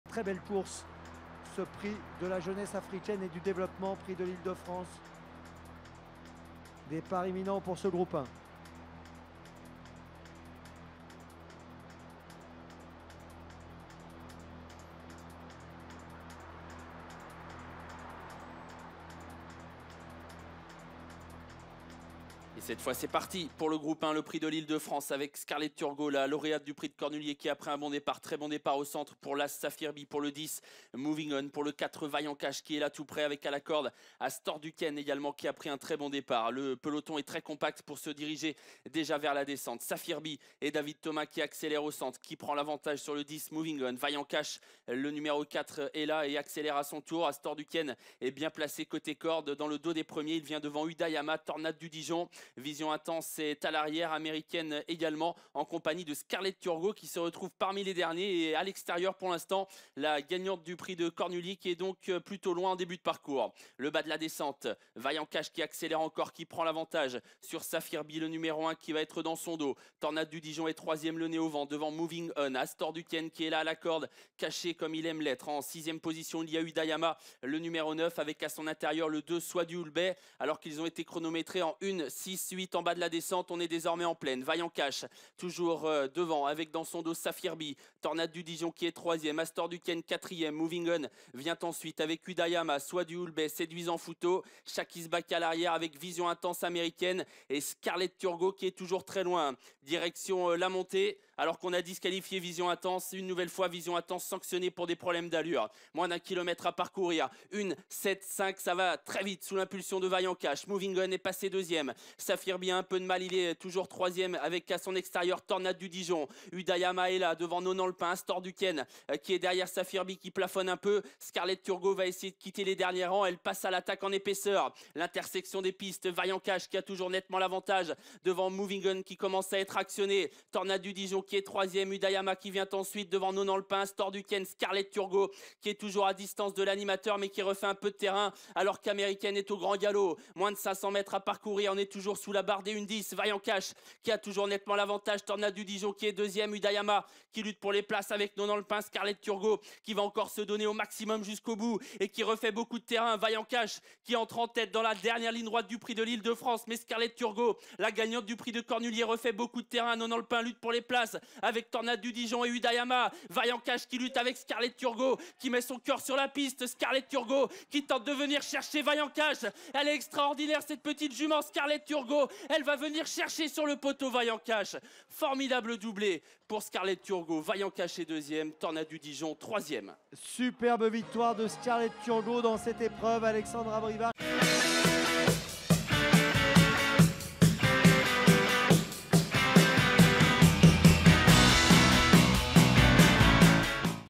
Prix de l'Ile de France 2016 - La Course
SCARLET TURGOT drivé par Alexandre Abrivard a remporté le Prix de l'Ile de France 2016, course au trot monté de Groupe 1 (le 7 février 2016 à Paris Vincennes)
2e : VAILLANT CASH
3e: TORNADE DU DIGEON
4e: UDAYAMA
5: NONANT LE PIN